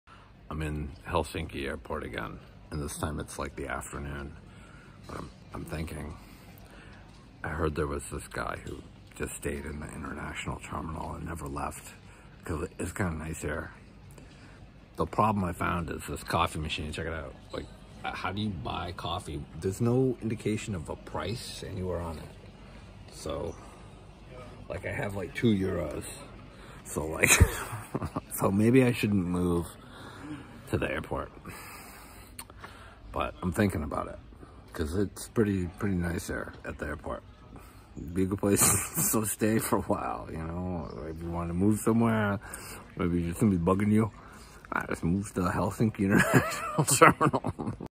Big Announcement! I am relocating to the International Terminal at Helsinki airport
It is way nicer than anywhere I have EVER been!